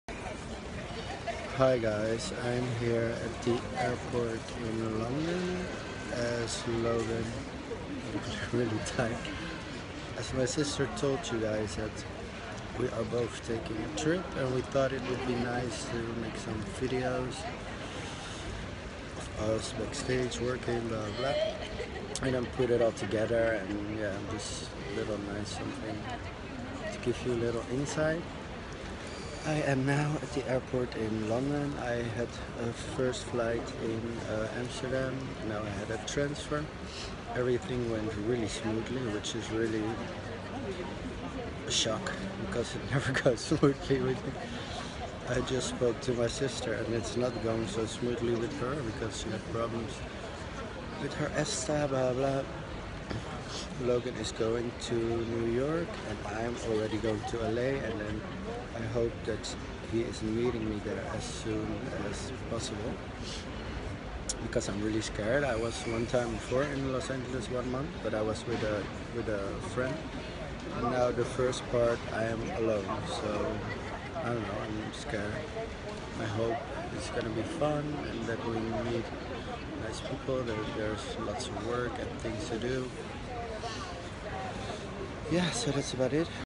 Joeri Alexander part 01 on tour 2017
Hello guys this is my bestie and gay siss Joeri Alexander. Together we do this tour so you will hear his side of his stories of our adventures! Hope you all like it! LM